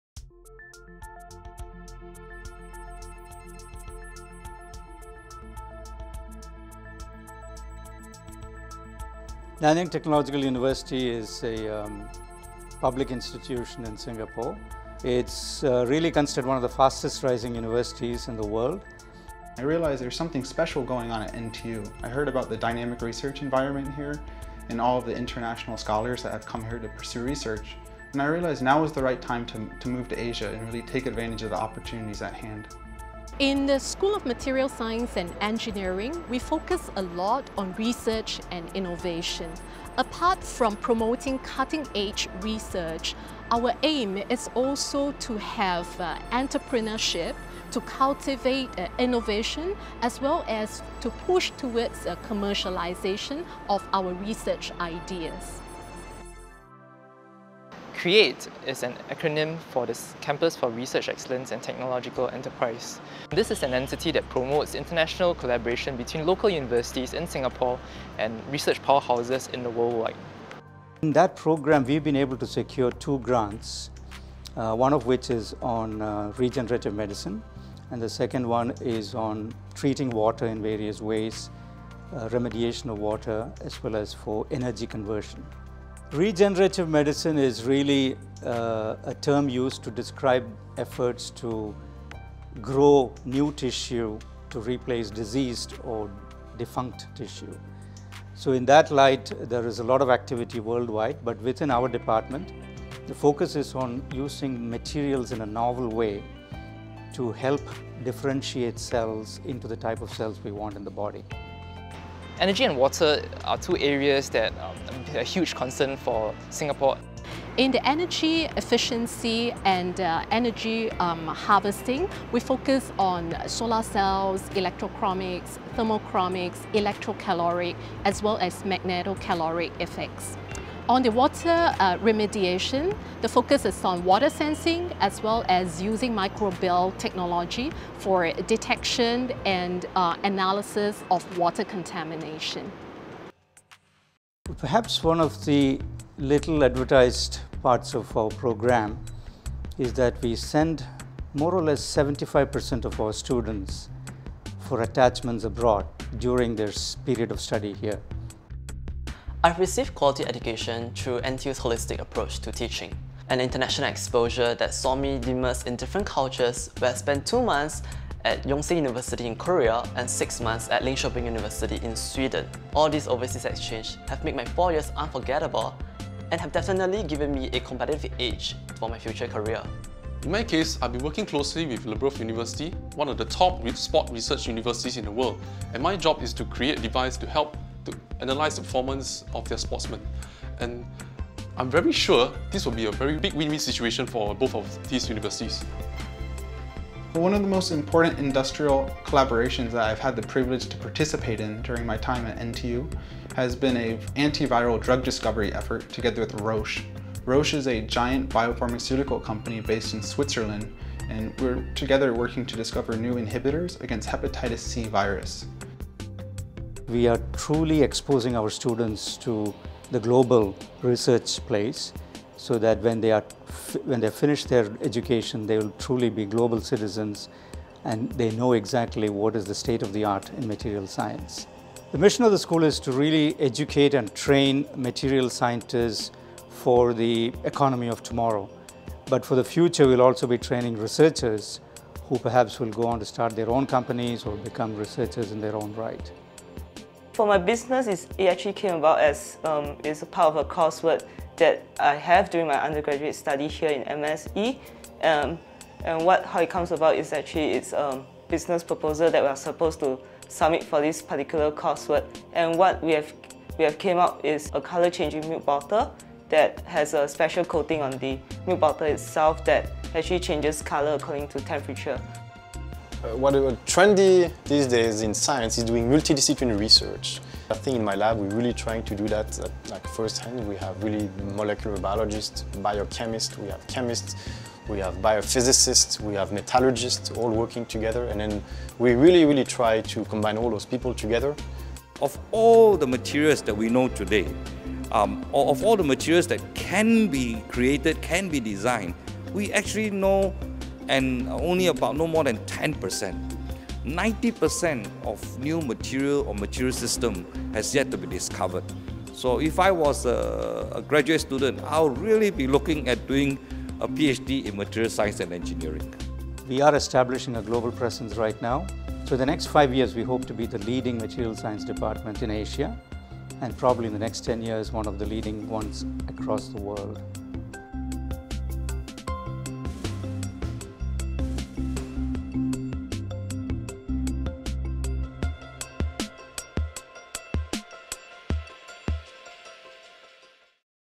Nanyang Technological University - Building a Global Reputation
Students at NTU's School of Materials Science & Engineering are immersed in international partnerships and collaborations. These aspiring scientists receive a rich and diverse education and are also trained as future entrepreneurs. MRS TV takes you to Singapore to one of the fastest rising Universities in the world.